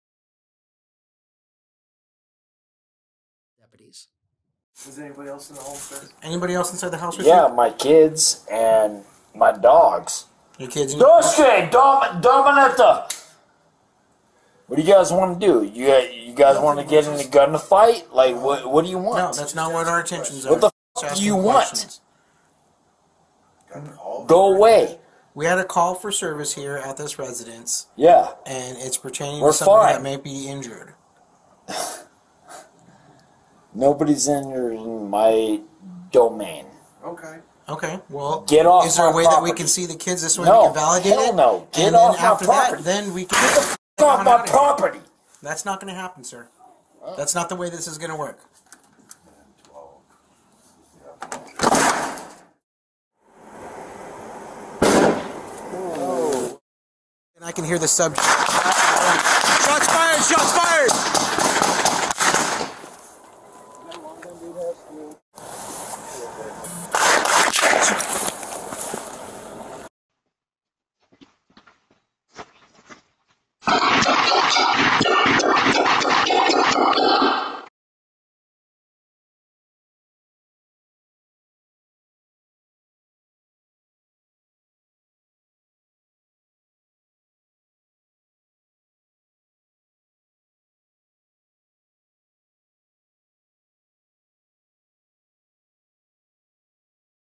Deputies sent domestic violence suspect to meet his maker after he shot at them with tactical rifle
On Tuesday, January 9, 2024, at 11:41 p.m., deputies assigned to the Hemet Sheriff’s Station responded to the 26300 block of Jepson Court in Hemet for a report of a physical altercation between family members where a female had sustained injuries.
Deputies arrived at the residence and contacted the suspect at the front door. The suspect was uncooperative and shut the door. While deputies were outside attempting to contact the involved parties, the suspect fired at them with an assault rifle from within the residence. The suspect then exited the residence wearing body armor and armed with the assault rifle. He fired at deputies a second time and a deputy-involved shooting occurred.
The suspect was struck by gunfire and deputies rendered medical aid until paramedics arrived. The suspect was transported to a local hospital where he was later pronounced deceased. The adult female victim was located at the residence and treated for non-life-threatening injuries at the scene. No deputies were injured during this incident.